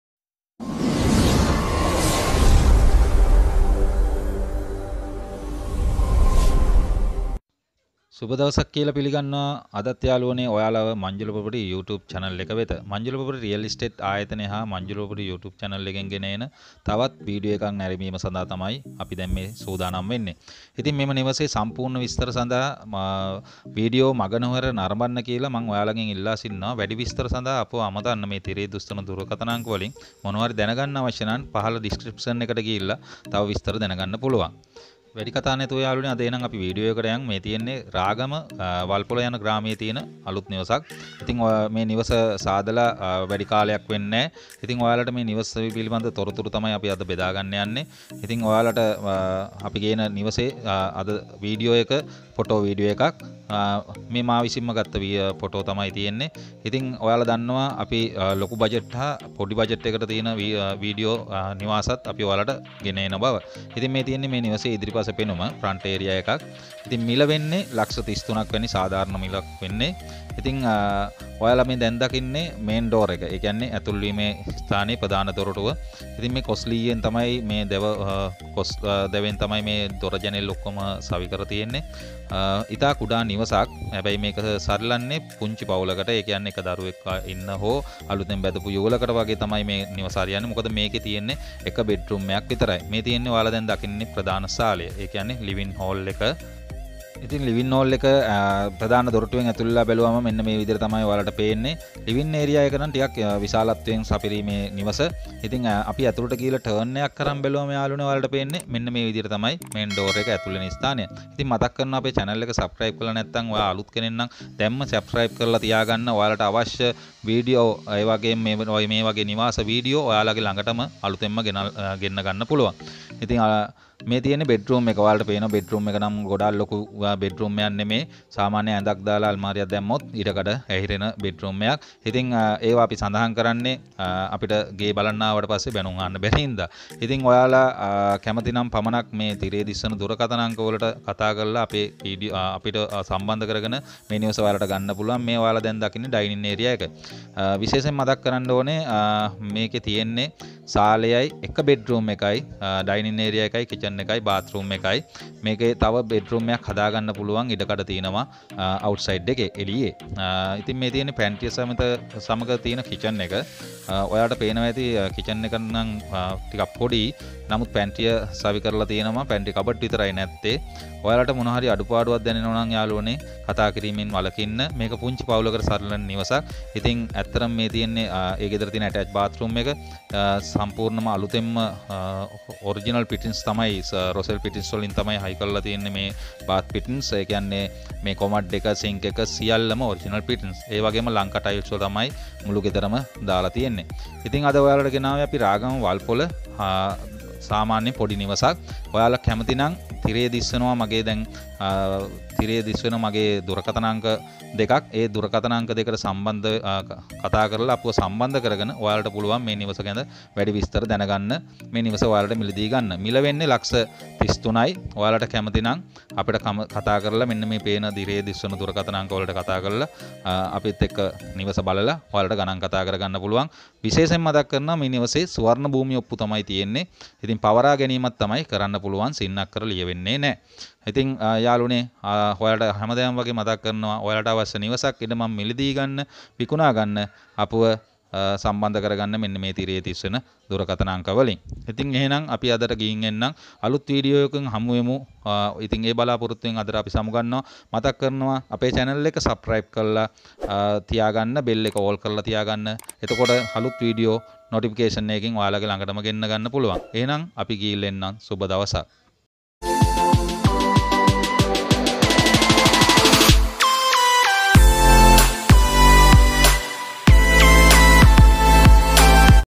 House_for_sale_in_srilanka | Luxury_house_for_sale | Manjula_property
detils Hous for sale in ragama

living
dining
1 bedroom
kitchen
Attached bathroom
4 perches land size

please call more detils this number 👇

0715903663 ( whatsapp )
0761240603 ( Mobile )